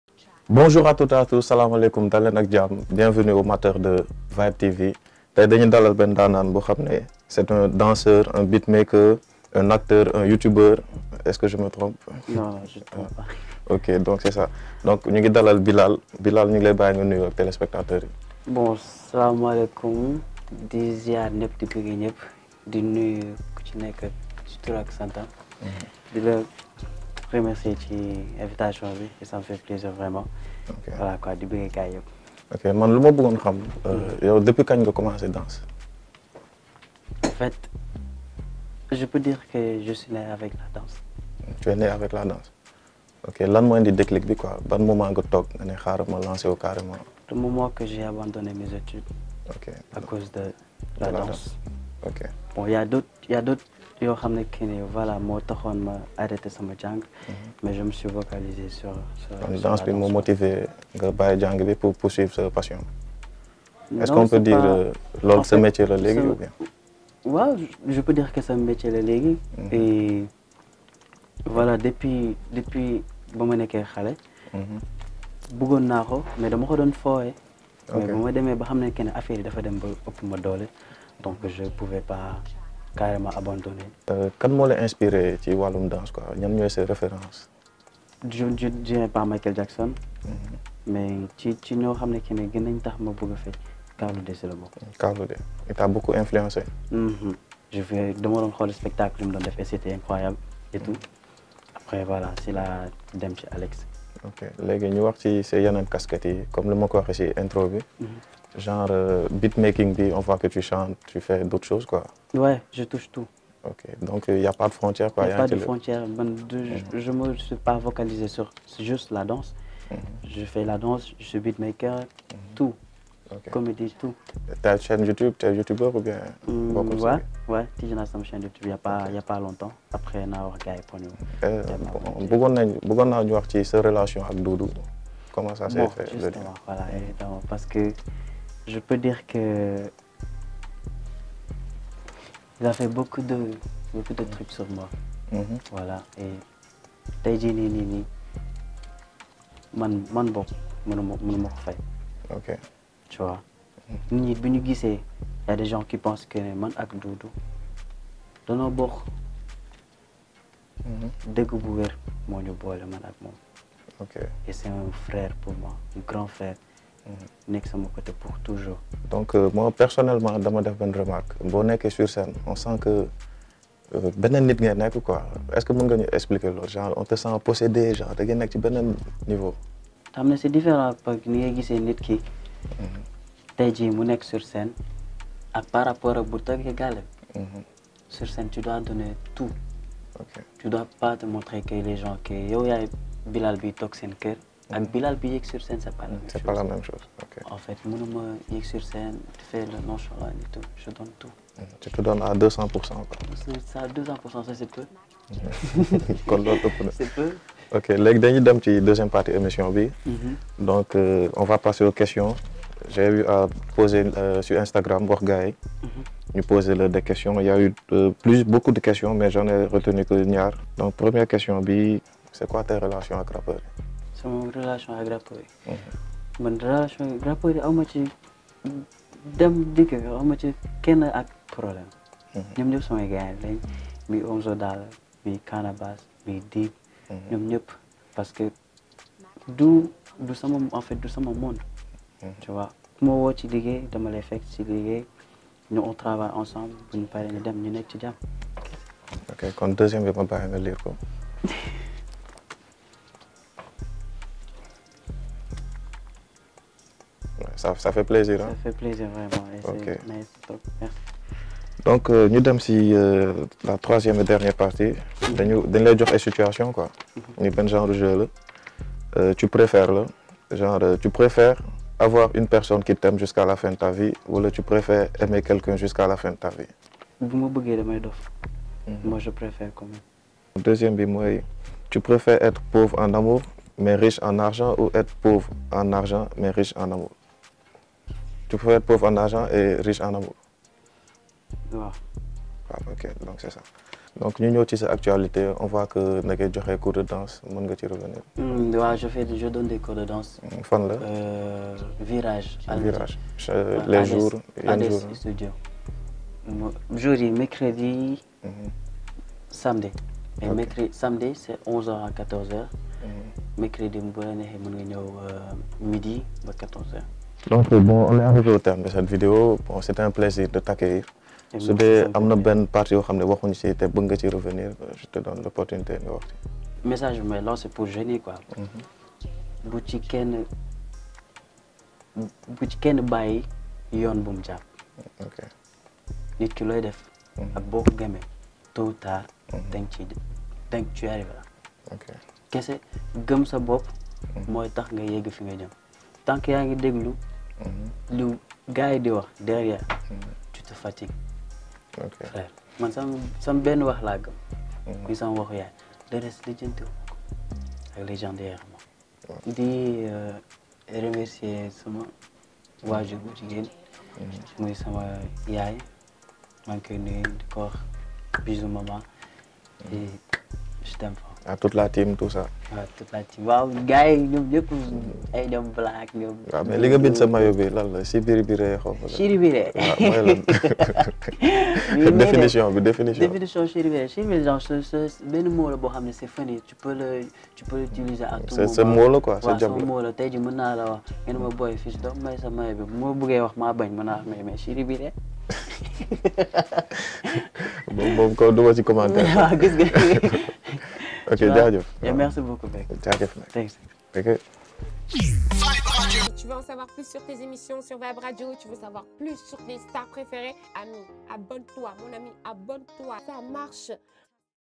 Bilal vs Math mes vidéos : "Ce que Dudu a fait pour moi..."
Math mes vidéos revient dans un nouvel épisode de Math versus et reçoit Bilal qui a tout dit sur ses métiers et sa relation avec Dudu.